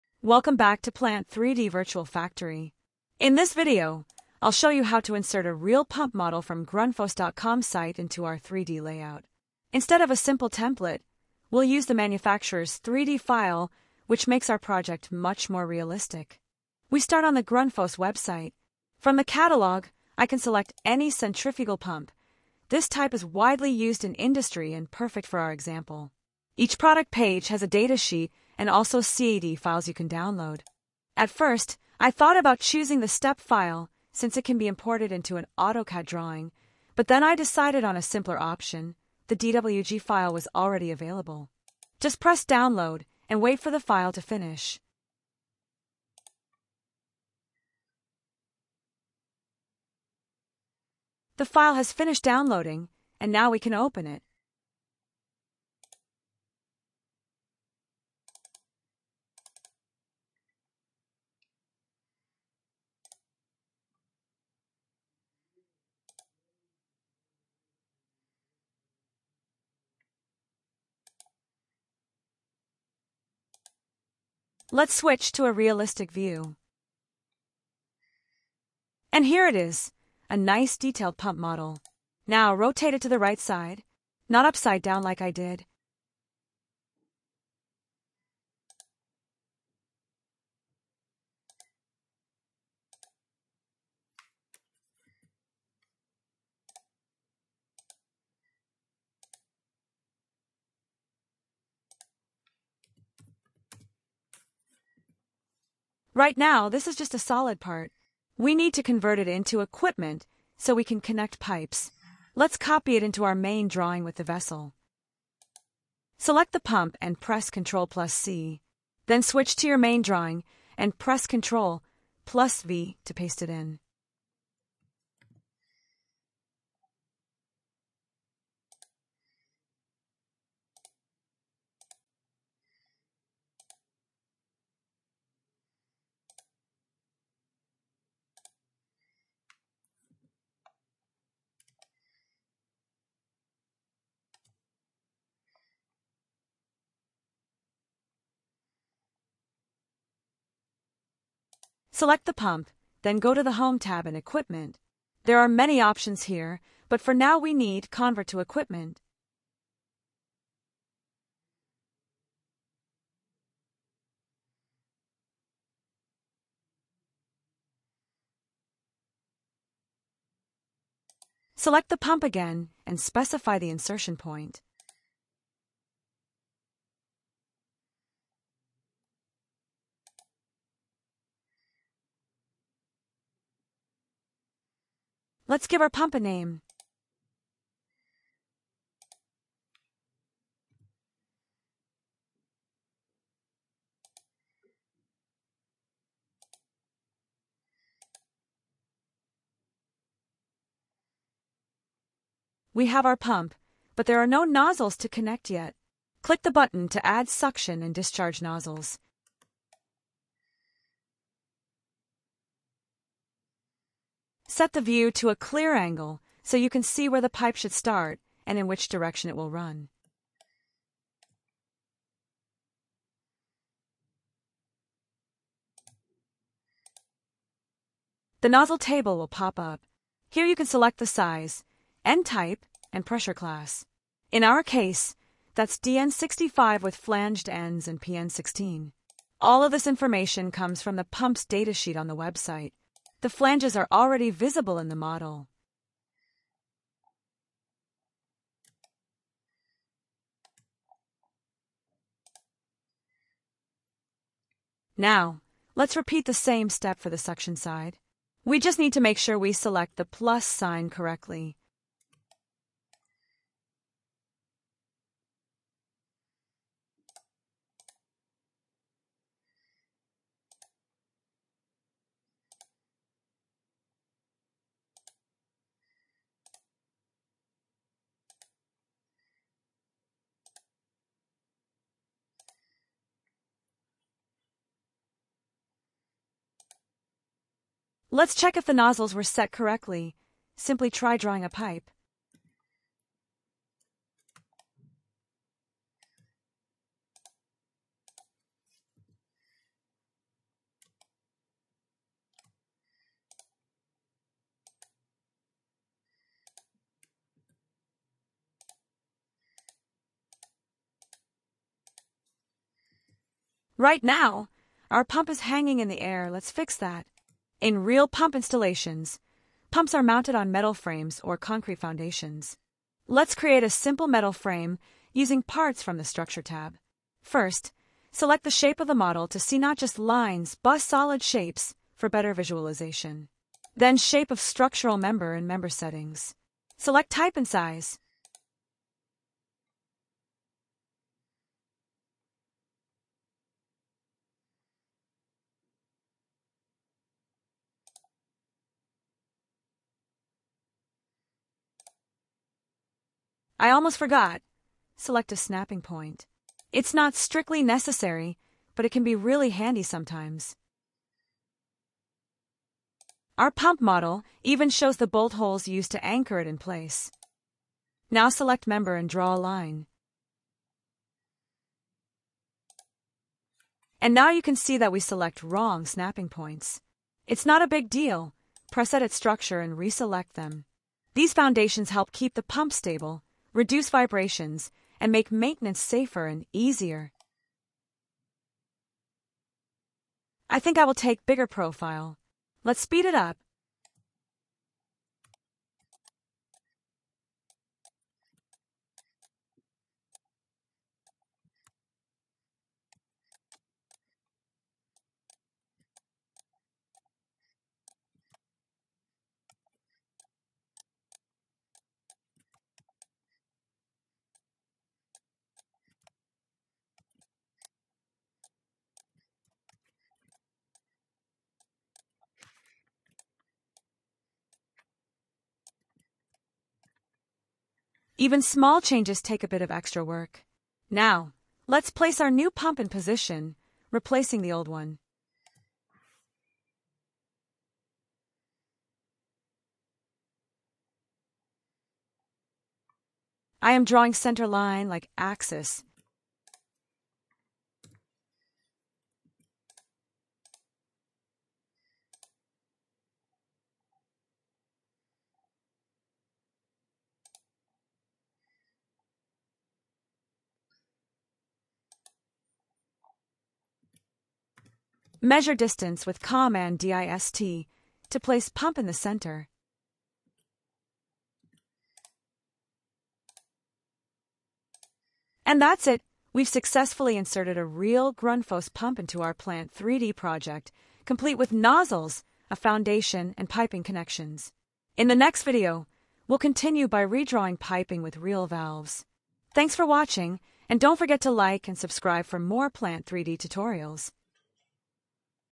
Insert Real Grundfos Pump into AutoCAD Plant 3D | Realistic Pump Modeling Tutorial
In this tutorial, I’ll show you how to insert a real Grundfos centrifugal pump into an AutoCAD Plant 3D project.
We’ll download the pump model directly from the Grundfos website, import it into Plant 3D, convert it into equipment, add nozzles, and place it on a foundation.

This workflow makes your design more realistic and closer to real-life installations compared to using standard templates. In this video we focus on preparing the pump as equipment — later it can be connected to piping.

What you’ll learn in this video:
-How to download CAD files from Grundfos
-How to import STEP/DWG pump models into Plant 3D
-How to convert solids into equipment with nozzles
-How to place a real pump model on a foundation
-How to prepare the pump for piping connections